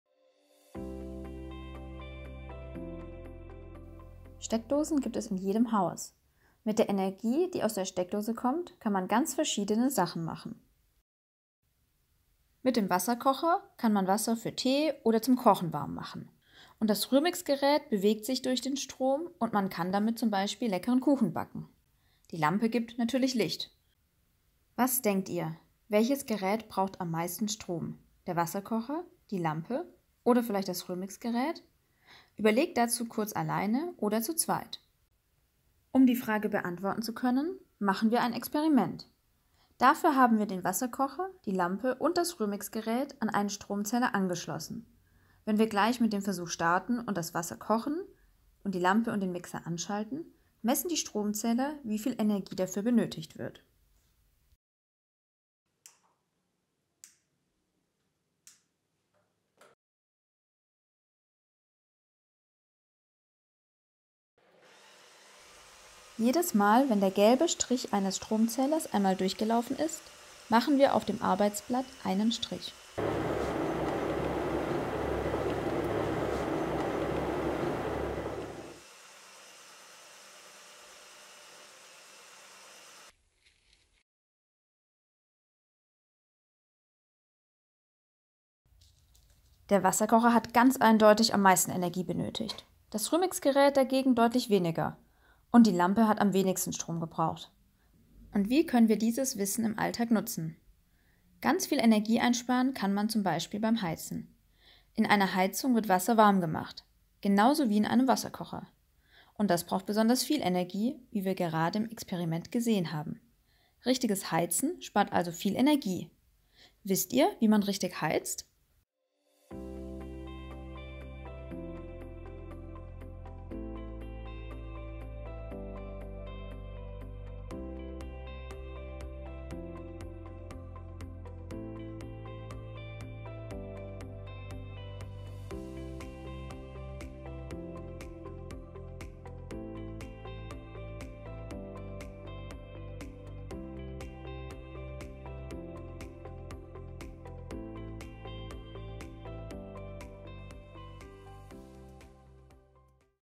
Stromzählexperiment - Schuljahr der Nachhaltigkeit
Das Video zeigt das Stromzähl Experiment aus dem Modul "Energie - schlau nutzen" des Projekts Schuljahr der Nachhaltigkeit Primar - Klimamodule, ein Projekt des Hessischen Umweltministeriums (HMUKLV), durchgeführt von ANU Hessen e.V. .